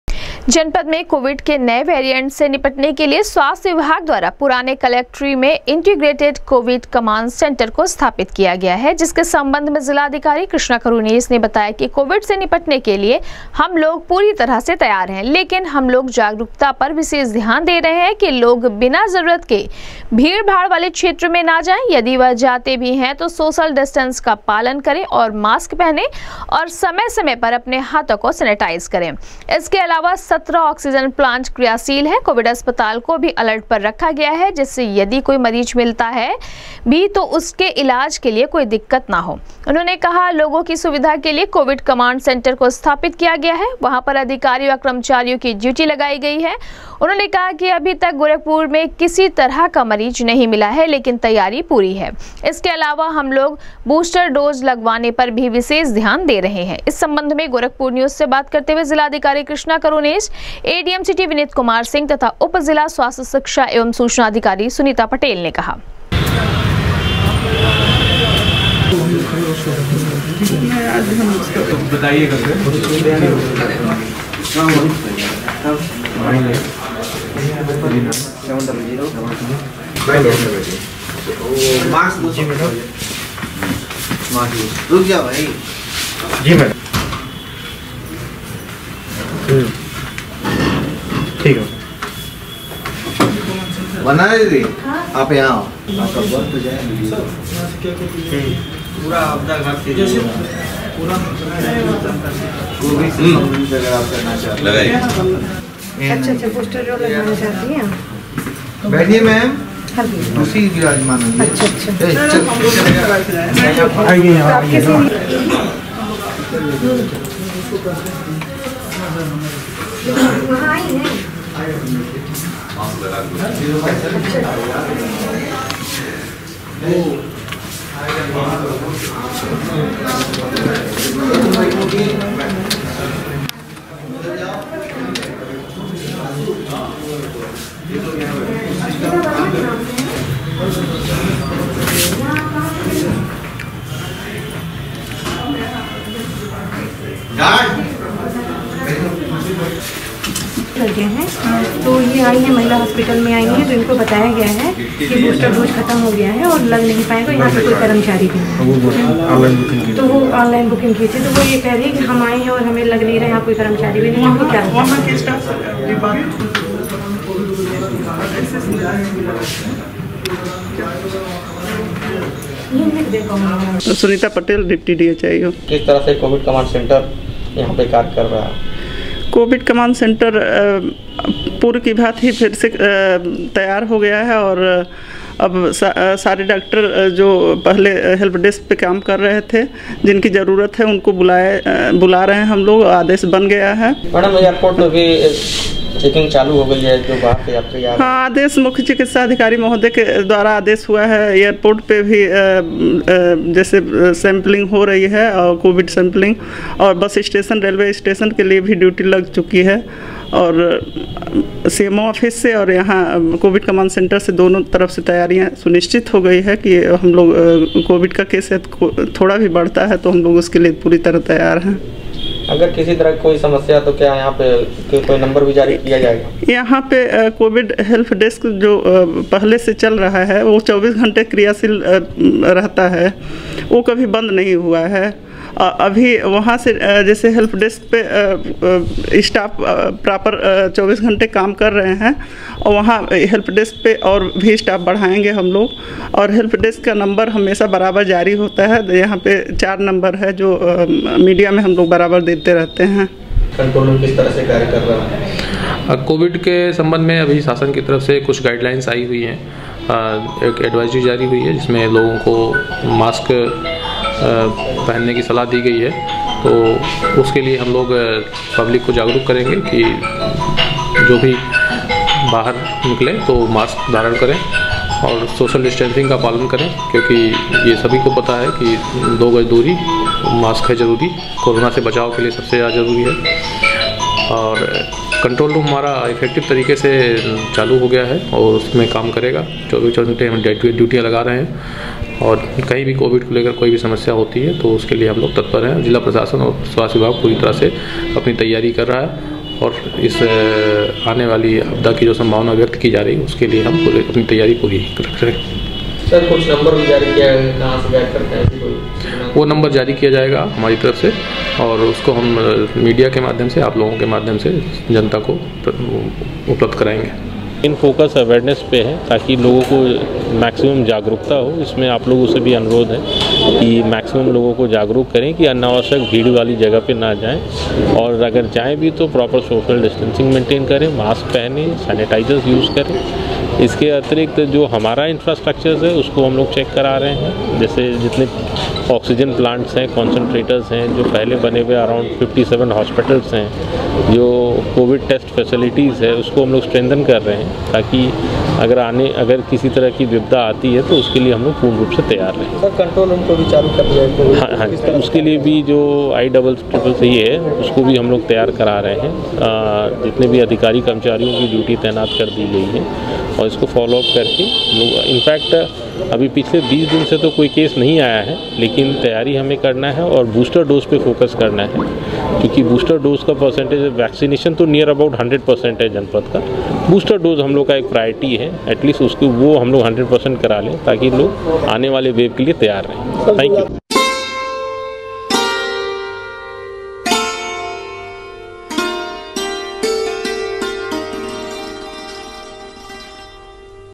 गोरखपुर में कोविड के नये वैरिएंट से निपटने के लिए पुरानी कलेक्ट्रेट में स्वास्थ्य विभाग के द्वारा
गोरखपुर में कोविड के नये वैरिएंट से निपटने के लिए पुरानी कलेक्ट्रेट में स्वास्थ्य विभाग के द्वारा इंटीग्रेटेड कोविड कंट्रोल रूम को किया गया स्थापित, 24 घंटे कर्मचारी कर रहे है ड्यूटी, 17 ऑक्सीजन प्लांट है क्रियाशील, हर तरह की परिस्थिति से निपटने के लिए है तैयार, जिलाधिकारी    कृष्णा करुणेश ने दी जानकारी